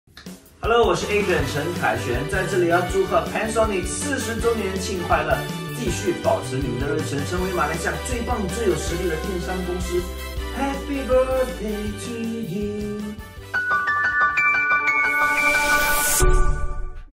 Pensonic 40th anniversary congratulatory message | Adrian Tan
Pensonic is celebrating 40th anniversary this year! These are the wishes from our beloved business partner, celebrities & friends.

Here’s Adrian Tan wishes for Pensonic’s Birthday and 40th Anniversary Celebration. Thank you Adrian Tan for your inspiring message and wishes for Pensonic’s success.